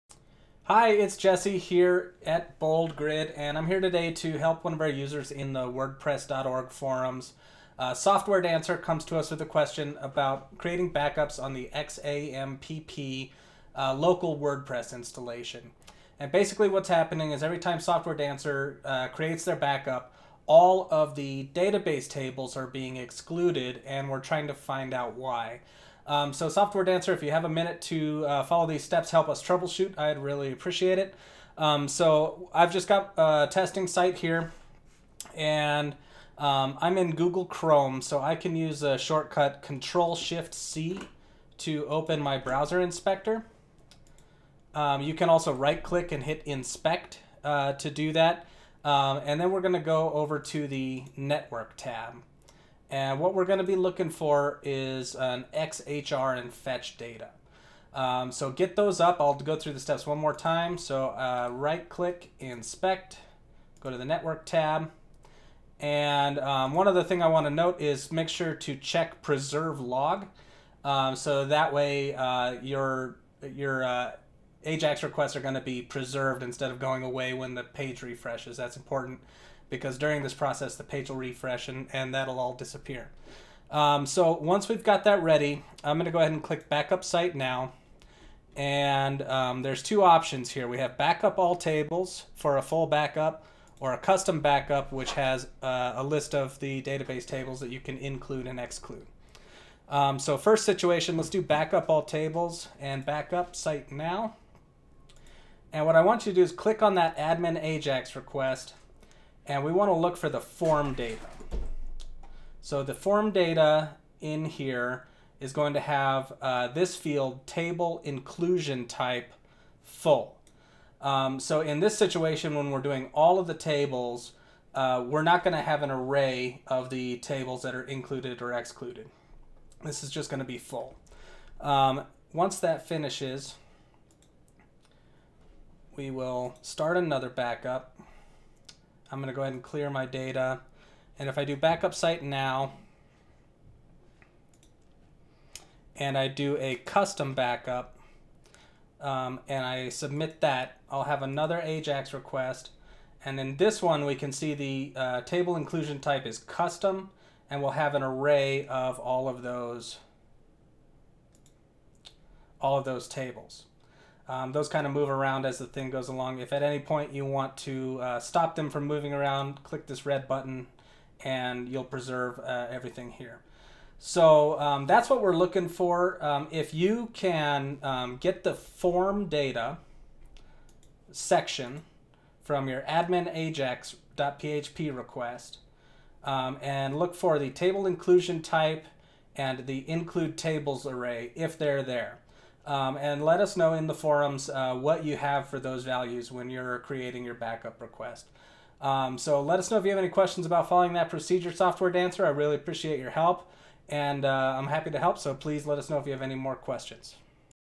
How to Check Submitted Form Data in Chrome Dev Tools
Total Upkeep user @softwaredancer is experiencing an issue where creating a backup in their local XAMPP WordPress installation excludes all of their database tables. Jesse walks you through how to use Chrome Dev Tools to inspect submitted form data, and what fields we expect to see when creating a backup.